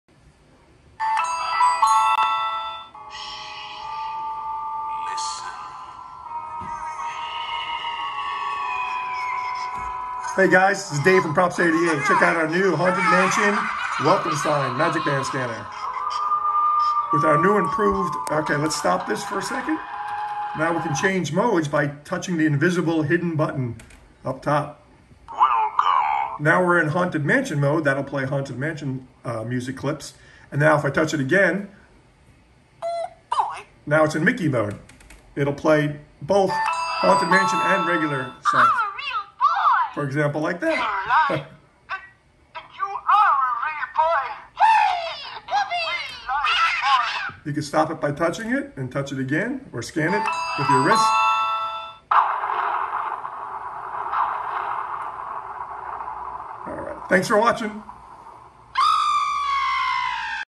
Haunted Mansion Entrance Sign Magic Band Scanner
** BRAND NEW HAUNTED DESIGN!! **

Welcome Foolish Mortals and Disney fans!  Now bring the magic of Disney inside your home!

• Two Magic Band Scanners in One! Haunted Mansion and Disney All!
• Laser cut Haunted Mansion and engraved text!
• Hidden TOUCH button on top of this sign! Touching hidden button allows you to:
  • Stop playing all music
  • Tap to put into Haunted Mansion Mode
  • Tap to put into Disney All Mode
• Now you can scan the mickey to activate! Built in proximity detector ( does not read magic bands unless it is on your wrist)
• New park like updated Copper Mickey Ball!
• HM MODE - Plays up to 27 Haunted Mansion Sounds/Music!
• Disney ALL MODE - Plays up to 150+ Disney Sounds/Music!
• Beautiful stone look scanner to match the color theme of the Haunted Mansion Ride, and of course the Copper painted Mickey!
• Front mounted speaker for better and louder sound!
• Rotating white LED ring shows first, followed by the GREEN ring of lights just like the real one!!
• Approximate OVAL 14inches x 12inches!
• USB cable included!
• USB Block included!
• Micro SD card included!
• Wall mountable

Sincerely,
Dave @ props88